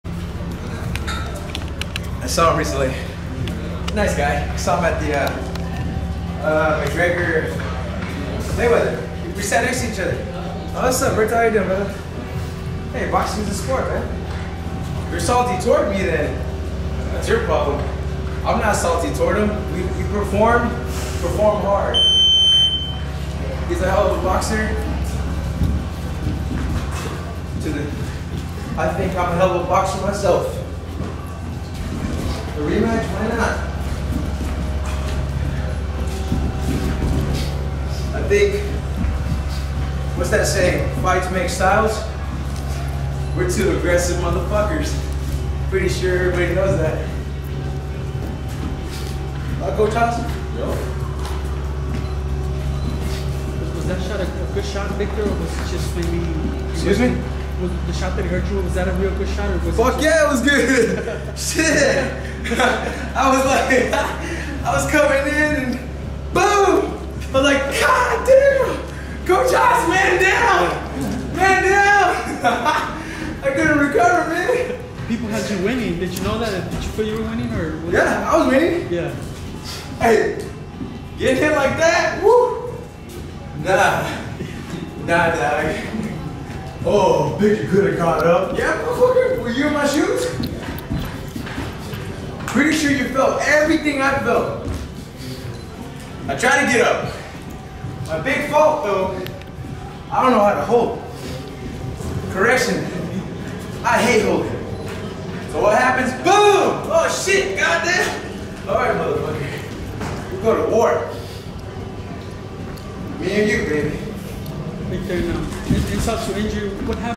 Victor Ortiz on rematch with Berto:" we are 2 aggressive motherf*ckers!!!! " - EsNews Boxing
Check out this video Victor Ortiz